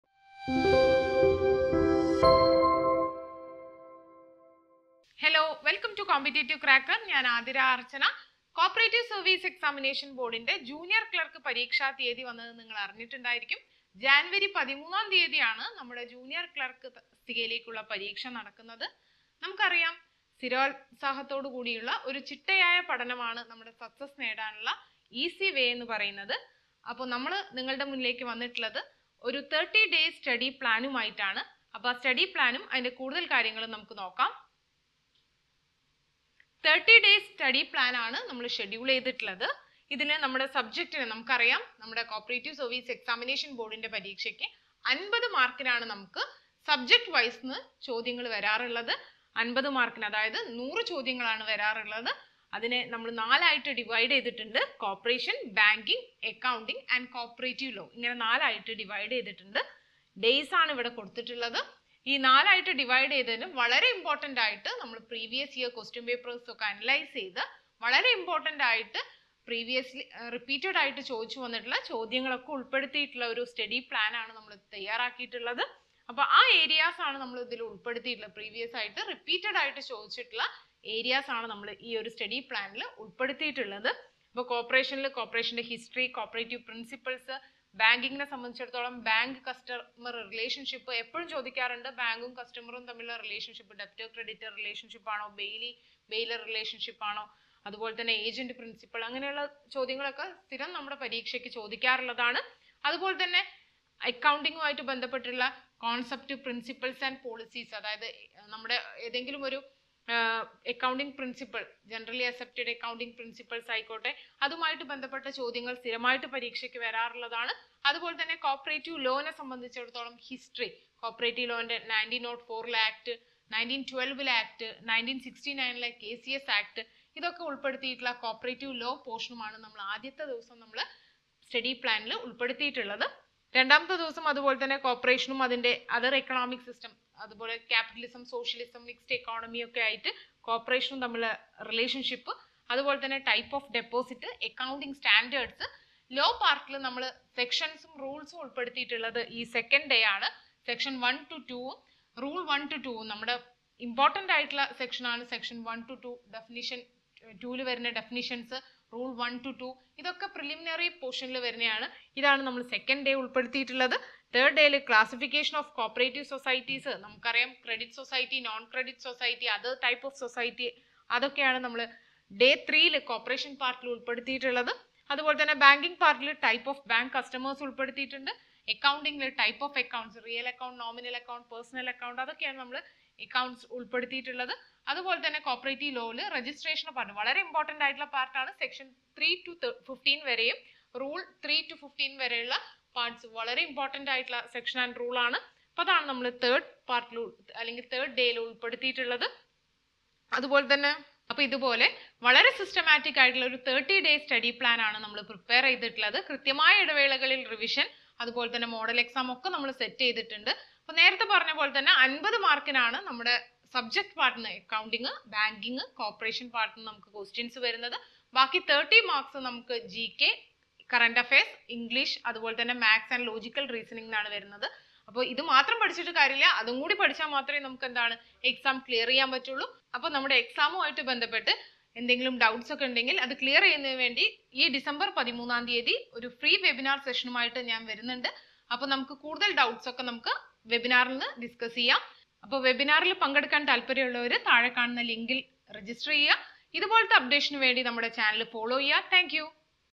CO-OPERATIVE BANK 30 DAYS STUDY PLAN
CO-OPERATIVE BANK EXAM നേടിയെടുക്കുക എന്നതാണോ നിങ്ങളുടെ ലക്ഷ്യം എവിടെ തുടങ്ങണം എങ്ങനെ മുൻപോട്ട് പോകണം എന്ന് സംശയങ്ങൾ ഉള്ളവരാണോ നിങ്ങൾ ?എങ്കിൽ പഠന സംബന്ധമായ എല്ലാ സംശയങ്ങൾക്കും ഉത്തരങ്ങളുമായി